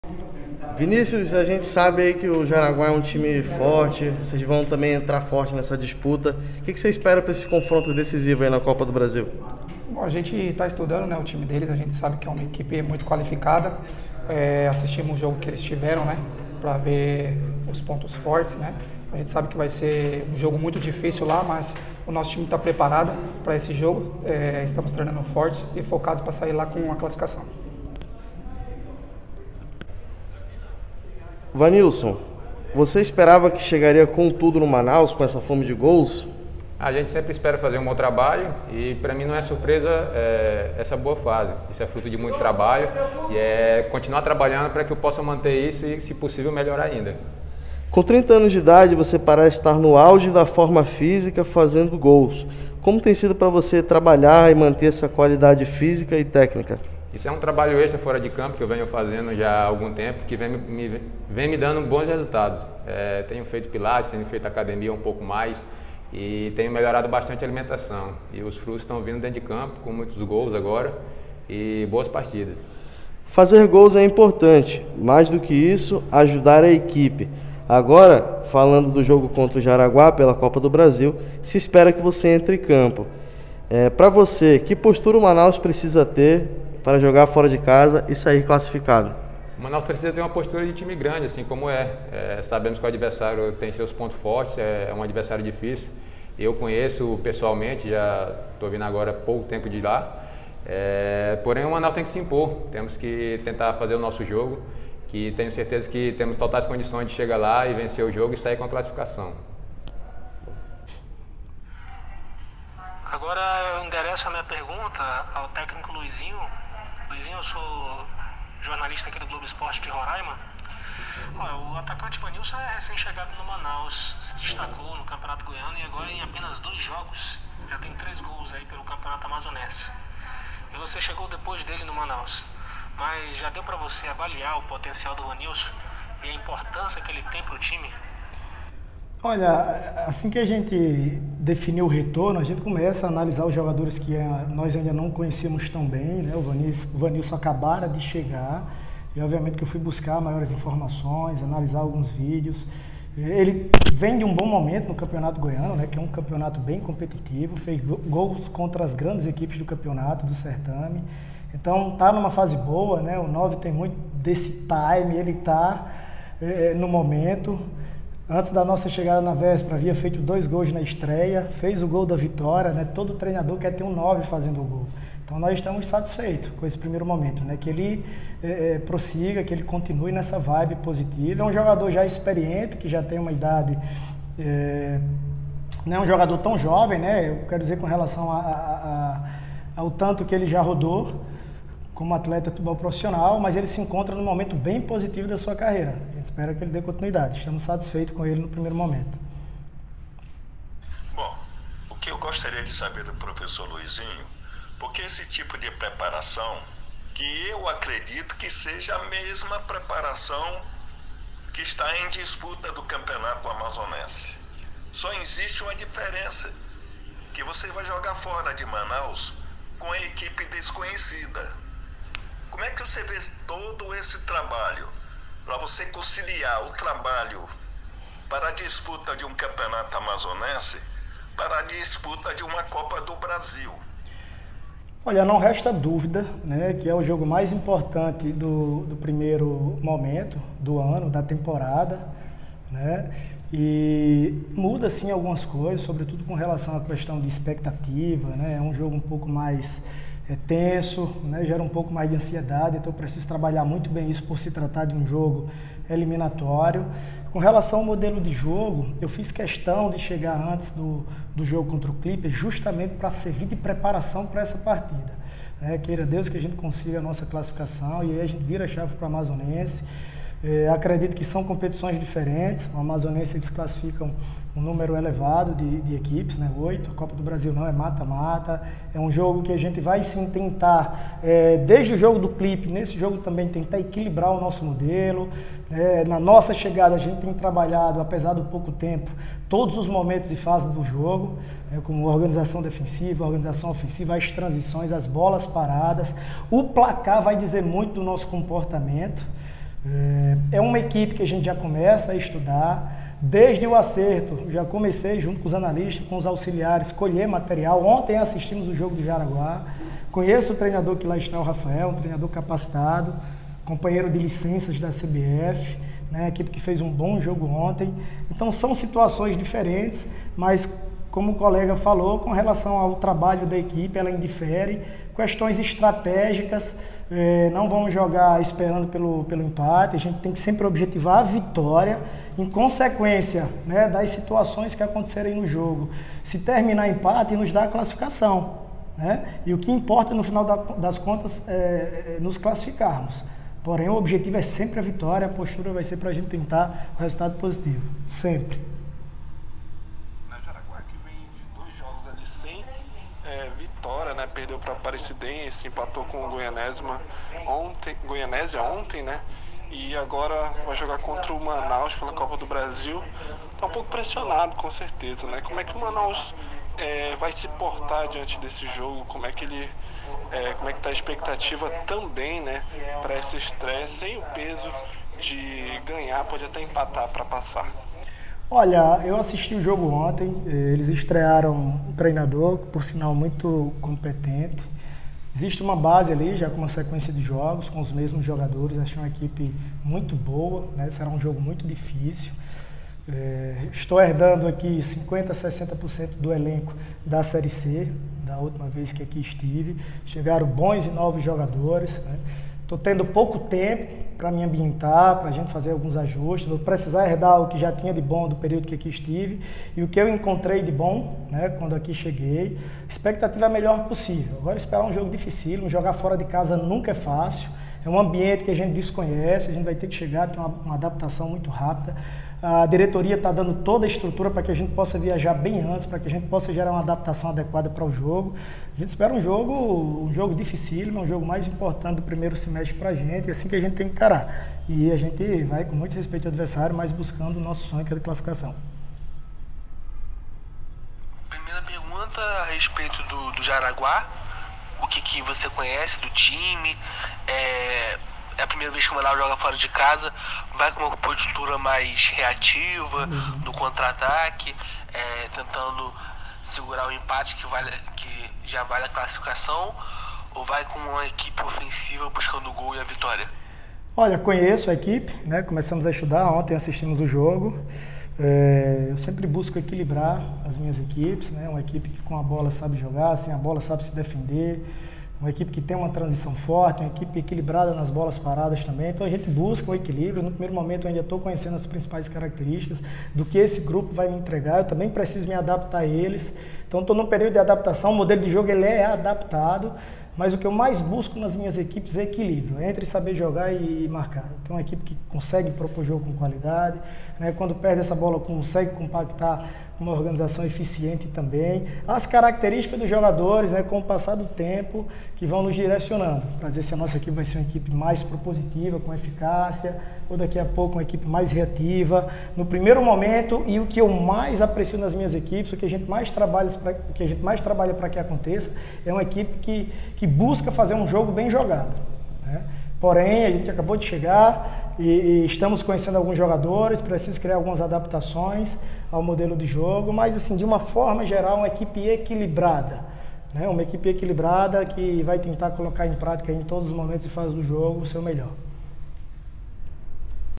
16ª Coletiva - Manaus2021​​​​ - Semana Copa do Brasil
Meia Vinicius 'Barba"
➡️ 0:01​​​​​​ - O que você espera desse confronto na Copa do Brasil?

Atacante Vanilson
➡️ 0:30 - Esperava que chegaria com tudo no Manaus?
➡️ 0:47 - No auge da sua forma física aos 30 anos, como tem sido trabalhar a parte física e técnica?
➡️ 1:13 - Pra você, que postura o Manaus precisa ter para arrancar a vitória na Copa do Brasil?

Técnico Luizinho Lopes
➡️ 1:55 - Vanilson é destaque no Manaus, com 3 gols em duas partidas. Já deu pra avaliar o potencial do jogador?
➡️ 3:22 - Como é a preparação para campeonatos diferentes?
➡️ 5:50 - Baseado nos resultados do Jaraguá, como o Manaus irá se comportar diante do adversário?
➡️ 7:20 - O que você conhece do Jaraguá?